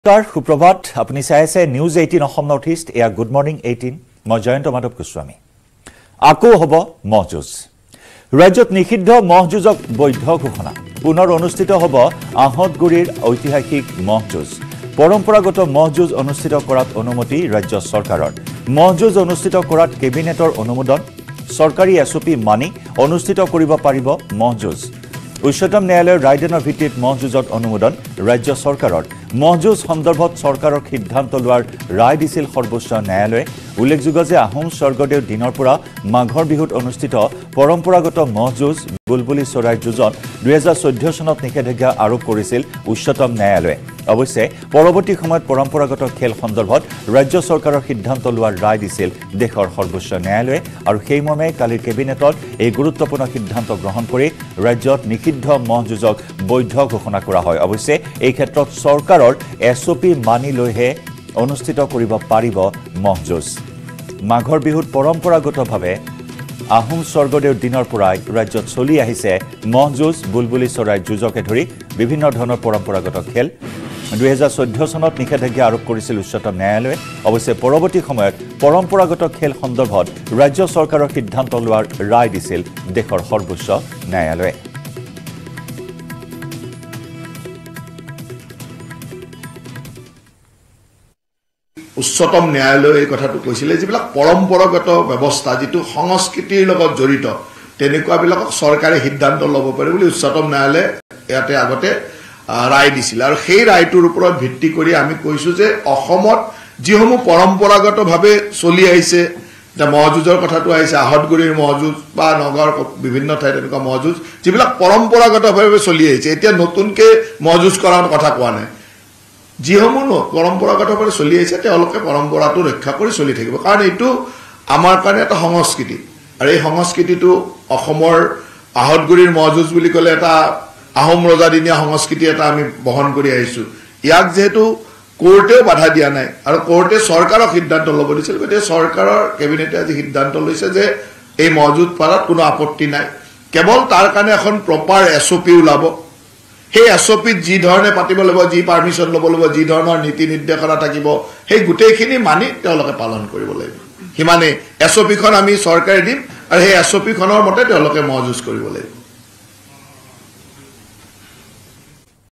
Assam Govt on Traditional Buffalo Fight : ৰাজ্যত নিষিদ্ধ ম’হ যুঁজক বৈধ ঘোষণা। Assamese News
Assam Govt on Traditional Buffalo Fight | ৰাজ্যত নিষিদ্ধ ম’হ যুঁজক বৈধ ঘোষণা।
পুনৰ অনুষ্ঠিত হ’ব আঁহতগুৰিৰ ঐতিহাসিক ম’হ যুঁজ।
পৰম্পৰাগত ম’হ যুঁজ অনুষ্ঠিত কৰাত অনুমতি ৰাজ্য চৰকাৰৰ।
ম’হ যুঁজ অনুষ্ঠিত কৰাত কেবিনেটৰ অনুমোদন।
চৰকাৰী SOP মানি অনুষ্ঠিত কৰিব পাৰিব ম’হ যুঁজ।
উচ্চতম ন্যায়ালয়ৰ ৰায়দানৰ ভিত্তিত ম’হ যুঁজত অনুমোদন ৰাজ্য চৰকাৰৰ।
ম’হ যুঁজ সন্দৰ্ভত চৰকাৰক সিদ্ধান্ত লোৱাৰ ৰায় দিছিল সৰ্বোচ্চ ন্যায়ালয়ে।
উল্লেখ্যযে আহোম স্বৰ্গদেউৰ দিনৰ পৰা মাঘ বিহুত অনুষ্ঠিত পৰম্পৰাগত ম’হ যুঁজ, বুলবুলি চৰাইজ যুঁজত ২০১৪ চনত নিষেধাজ্ঞা আৰোপ কৰিছিল উচ্চতম ন্যায়ালয়ে। অৱশ্যে পৰৱৰ্তী সময়ত পৰম্পৰাগত খেল সন্দৰ্ভত ৰাজ্য চৰকাৰৰ সিদ্ধান্ত লোৱাৰ ৰায় দিছিল দেশৰ সৰ্বোচ্চ ন্যায়ালয়ে।